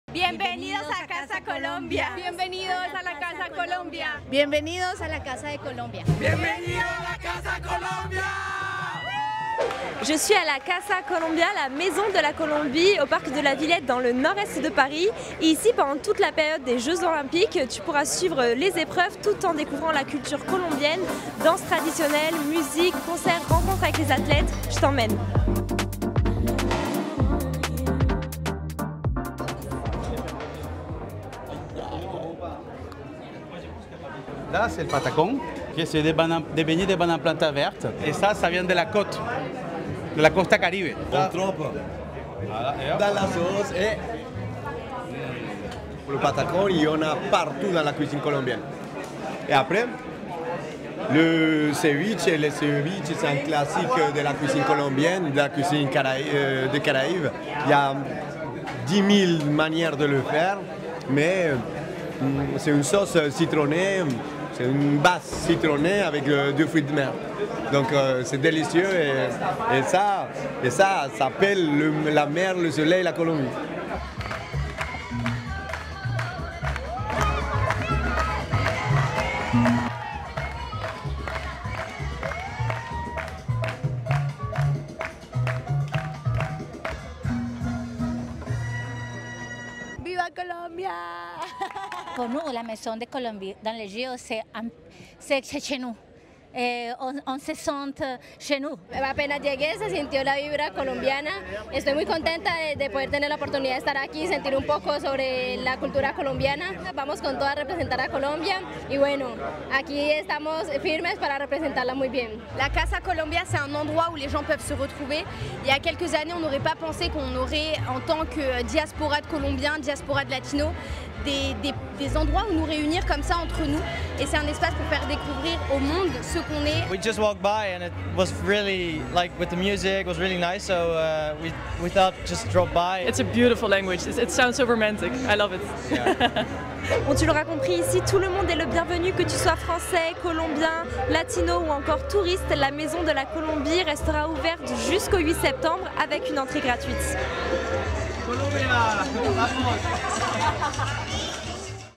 En París, la Casa Colombia celebra la cultura colombiana durante los Juegos Olímpicos
Los Juegos Olímpicos comenzaron oficialmente el viernes 26 de julio con la Ceremonia de Inauguración. Pero la fiesta también es internacional. Las casas de los comités olímpicos nacionales se han instalado en París. Su objetivo: promover su cultura a través de este gran acontecimiento deportivo. Descubra la Casa Colombia, situada en el Parque de la Villette, que mezcla encuentros con atletas, cocina, danzas y muchas más actividades inspiradas de Colombia.